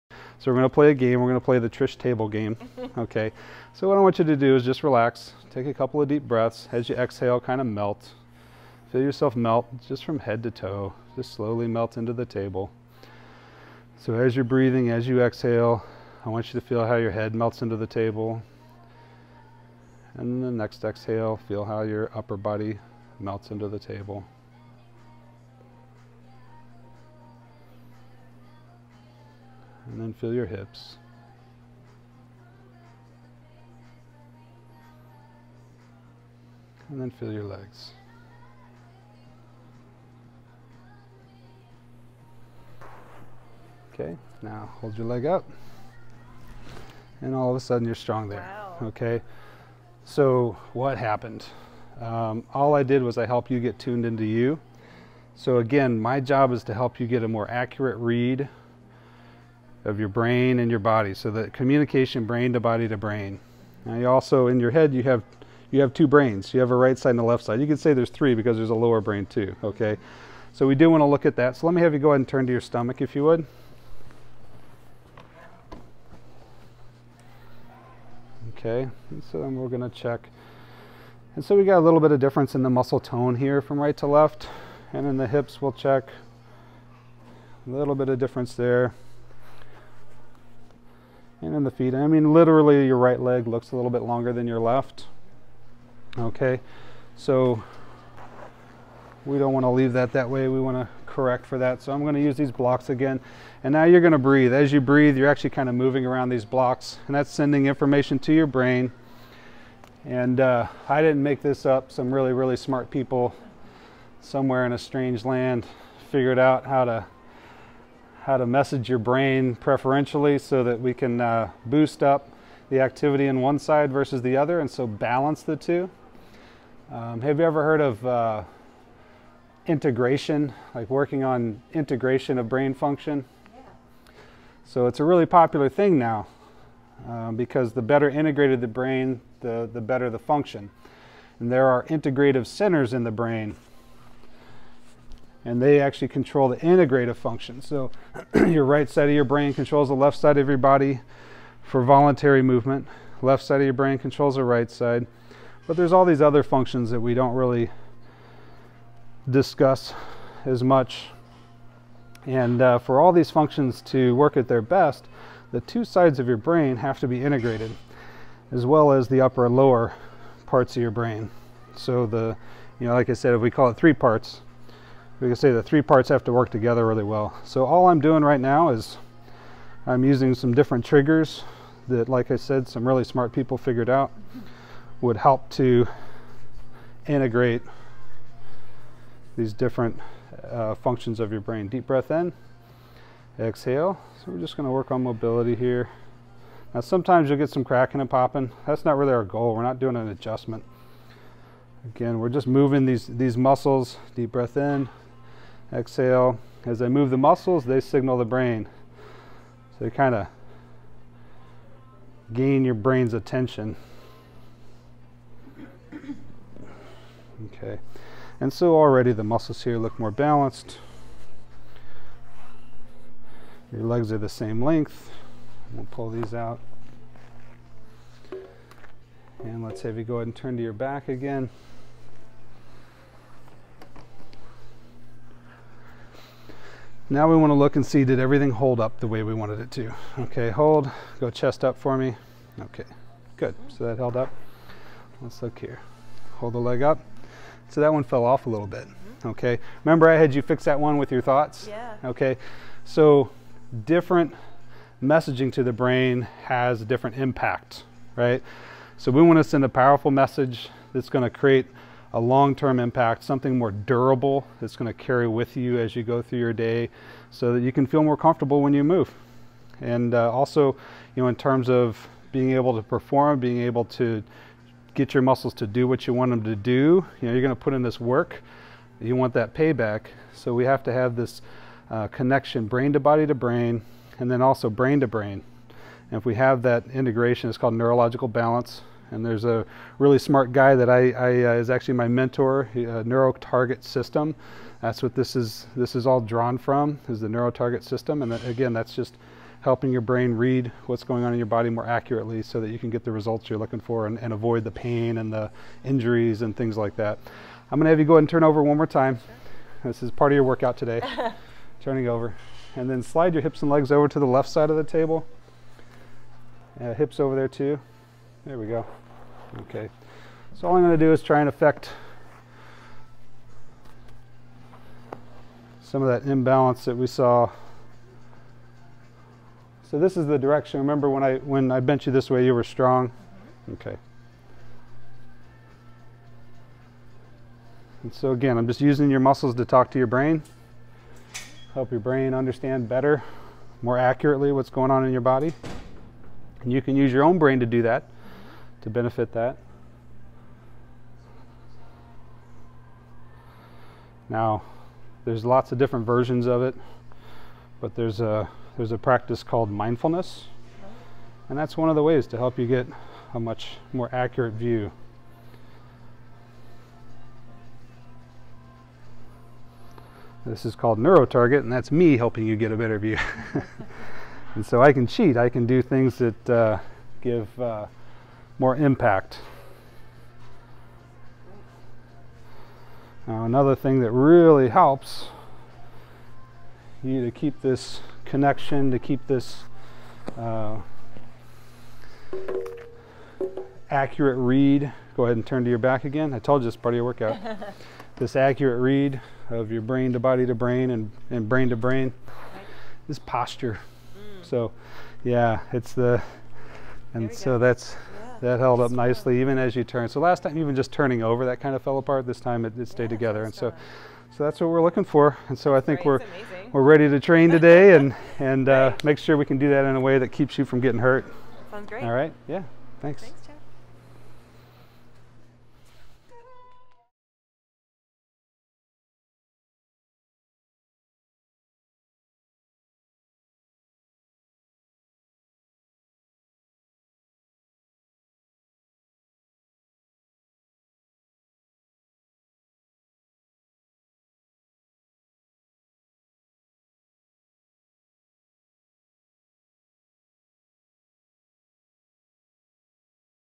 NeuroTarget Assessment with Jeff Powell - part 3
Among Jeff Powell's many qualifications is a level 2 certification in a form of functional neurology known as The NeuroTarget System.

Put simply, NeuroTarget is about correcting imbalances in the nervous system in order to treat and prevent injury.

Here, Jeff introduces The NeuroTarget System to one of our own.